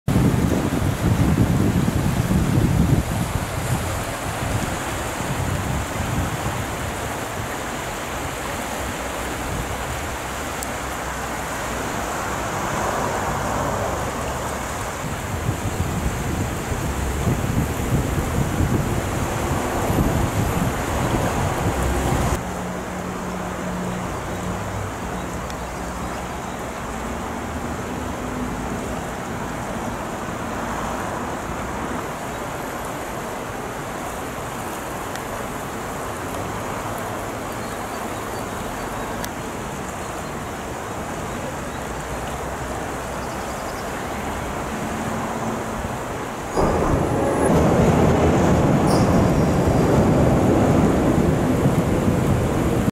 Úprava nábřeží u Svratky v Brně
Úprava nábřeží u Svratky pod železničím mostem kousek před nádražím Brno hlavní... Právě tento most byl nedávno při úpravách poškozen.
Natočeno v rámci projektu Ireportér ČT.